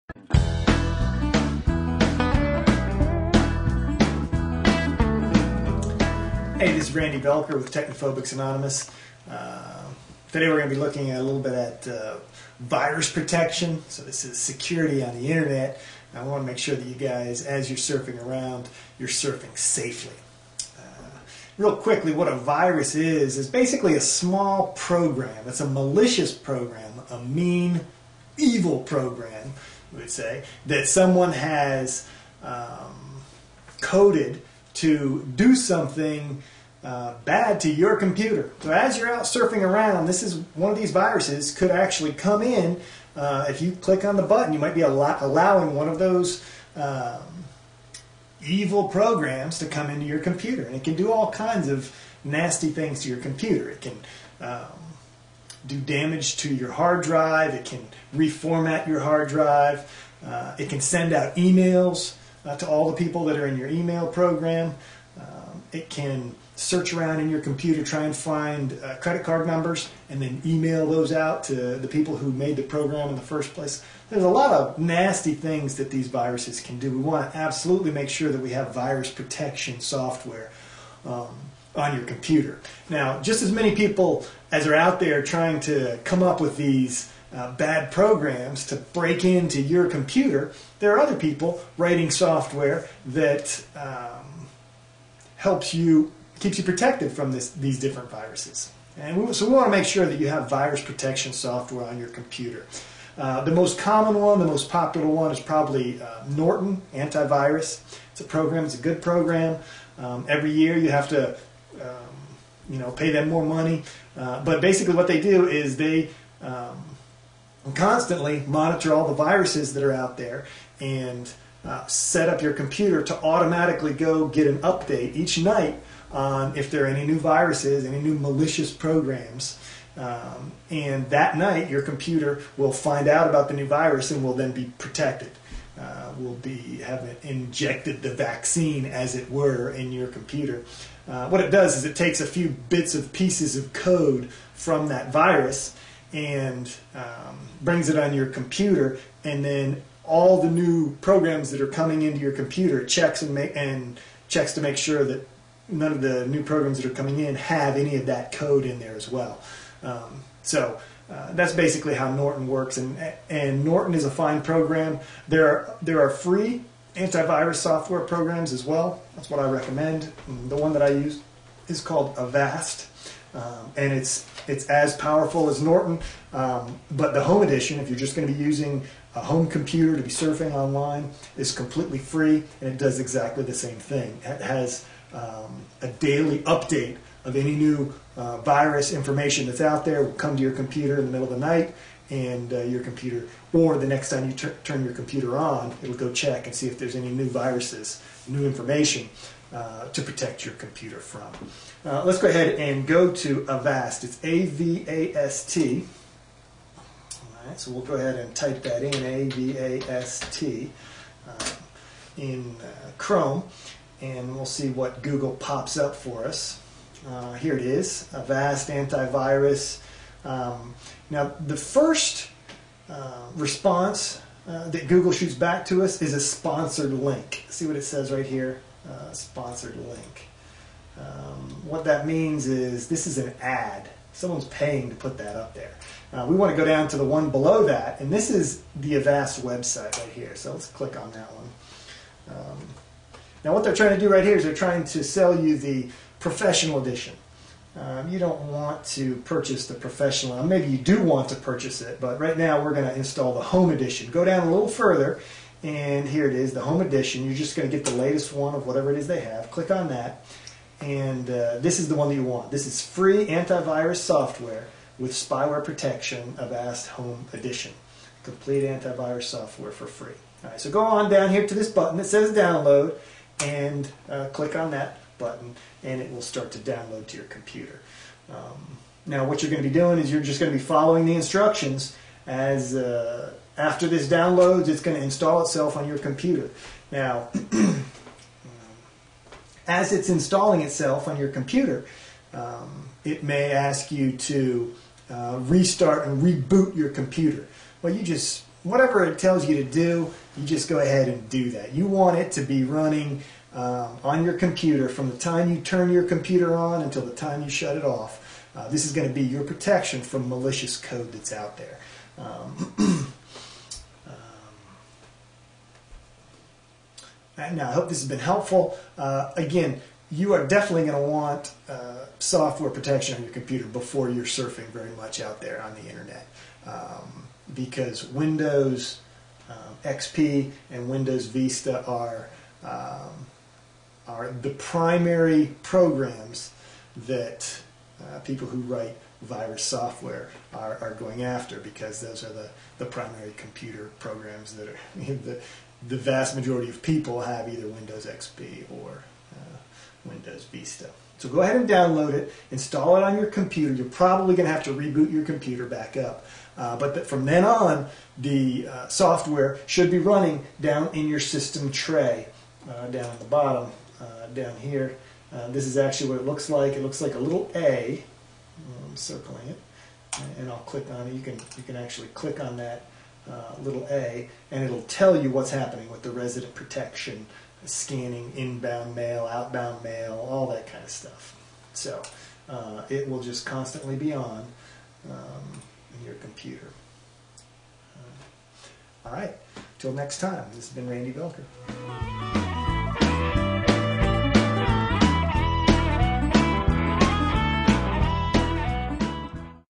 Viruses: Basic computer skills: Video Training (14 of 24)
Enjoy these Video Tutorials on Basic Computer skills.  Improve your productivity, your skills AND your confidence!  Perfect
for Beginners, Boomers, and Seniors. Virus Protection.